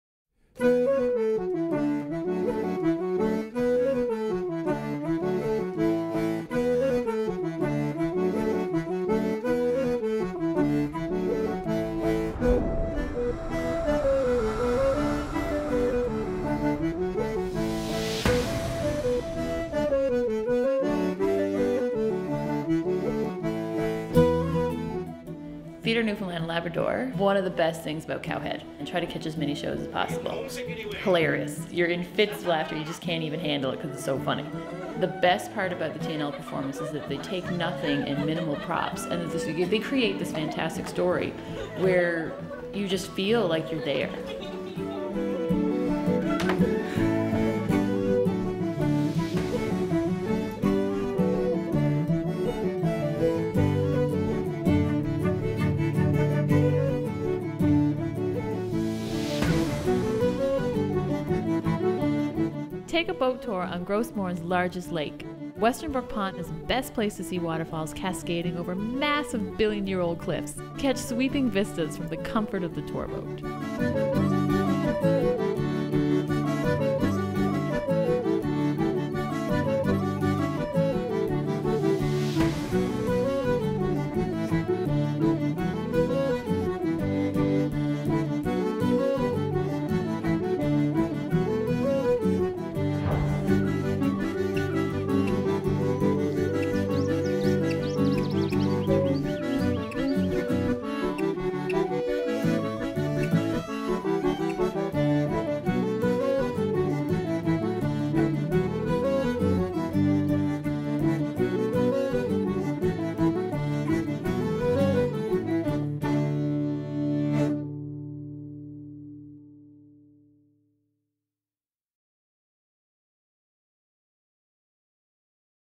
Gros Morne North
A great new video of experiences and accommodations in the north section of Gros Morne National Park (St. Paul's, Cow Head).